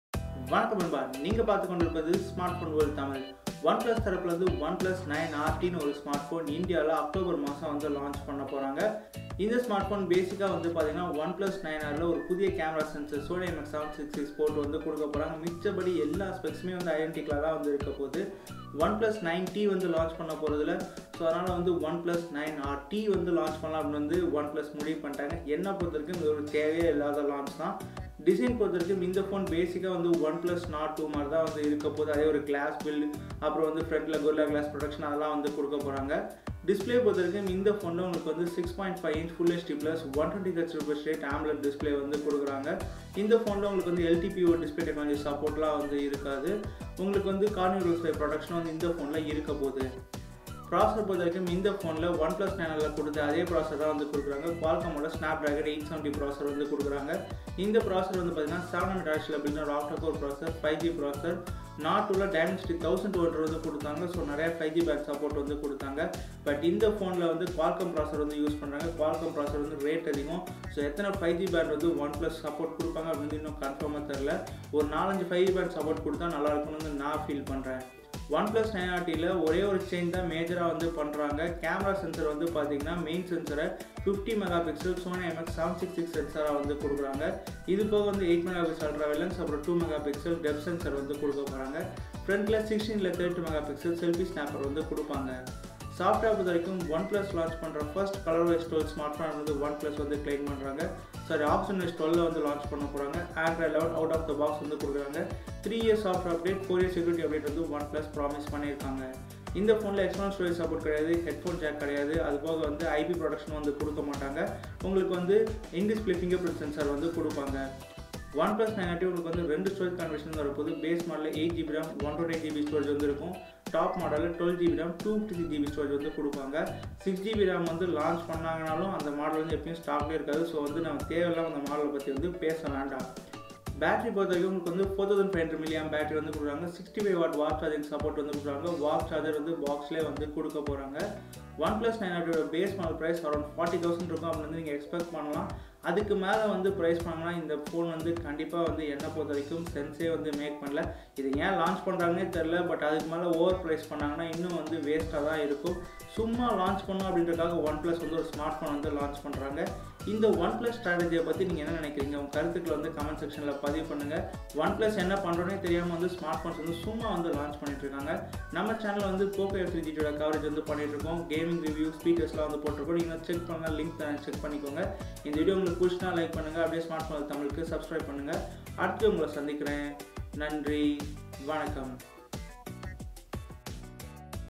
OnePlus 9RT specs tamil - Sd 870 | 50MP at 40k - OnePlus 9RT 5G price launch date in India tamil
OnePlus 9RT Tamil specs
     OnePlus 9RT 5G Specs, full details and price revealed in Tamil. OnePlus 9RT 5G Specifications include 6.5" FHD 120Hz Amoled display, 50MP Triple camera, 65W fast charge and 4500 mAh Battery. All above info is based on OnePlus 9RT 5G leaks and rumors. OnePlus 9RT 5G processor will be the Mediatek Dimensity 720 7nm octacore processor. 1Plus(1+) 9RT camera system will use 50MP Sony IMX 766 camera sensor. OnePlus 9RT 5G launch date in India is expected to be in October 2021. OnePlus 9RT india price will start from Rs.39,999. What are your thoughts about Samsung OnePlus 9RT price in India, share your thoughts on comments section!!